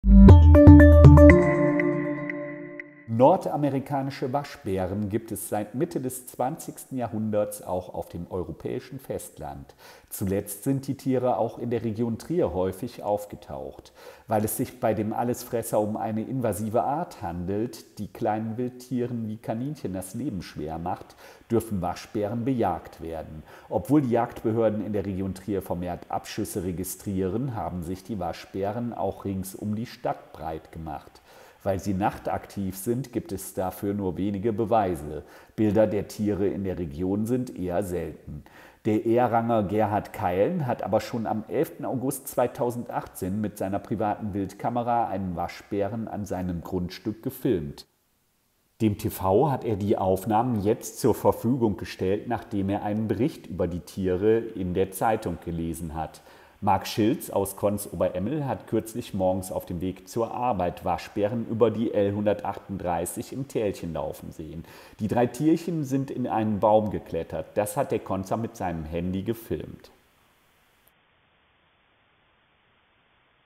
Waschbären kommen in die Region Trier
Nordamerikanische Waschbären leben schon länger in Mitteleuropa und seit einiger Zeit auch in der Region Trier. Das wird für kleinere Wildtiere zum Problem. Gesichtet werden die Tiere trotzdem selten. Ein Mann aus Trier-Ehrang hat dem TV jetzt aber Bilder seiner privaten Wildkamera zur Verfügung gestellt, die zeigen, dass Waschbären auch in der Stadt Trier unterwegs sind. Ein Oberemmeler hat drei Tiere im Tälchen aufgenommen.